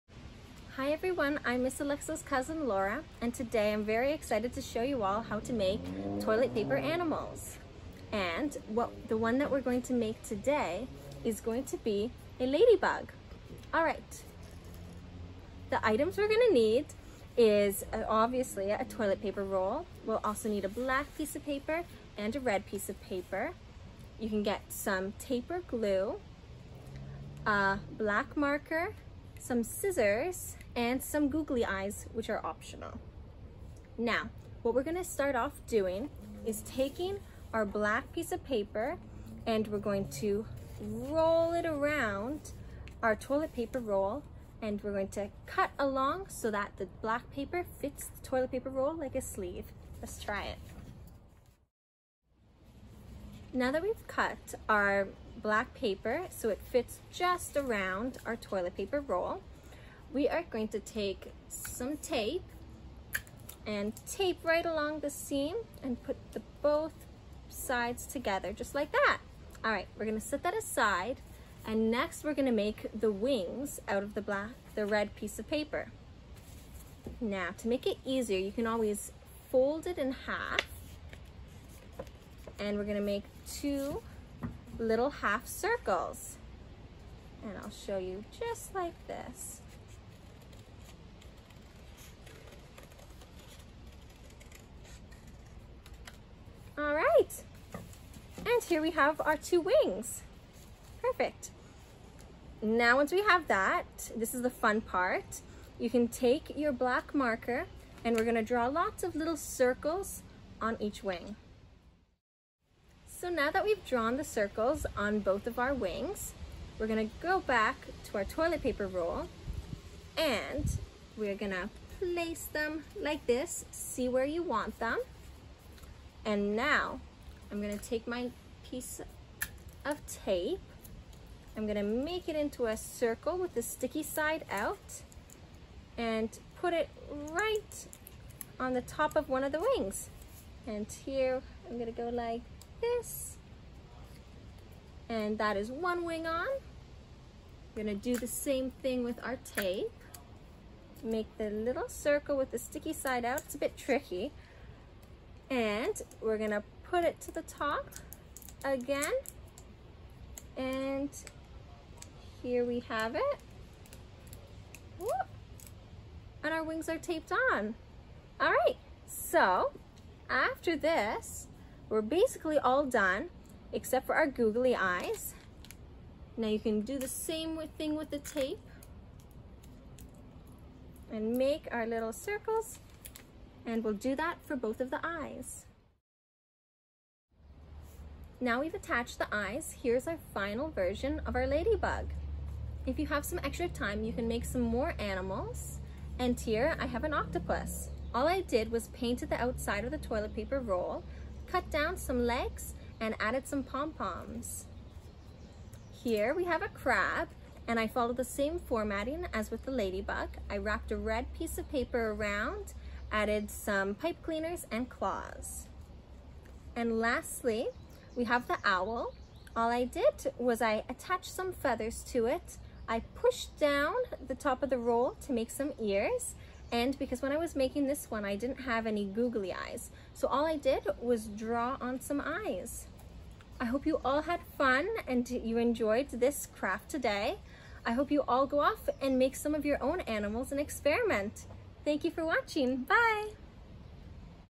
Toilet paper animals
Easy paper animal craft from toilet rolls

Items needed:
- Toilet paper rolls
- 1 red + 1 black paper
- Tape/Glue
- Marker
- Scissors
- Two googly eyes (optional)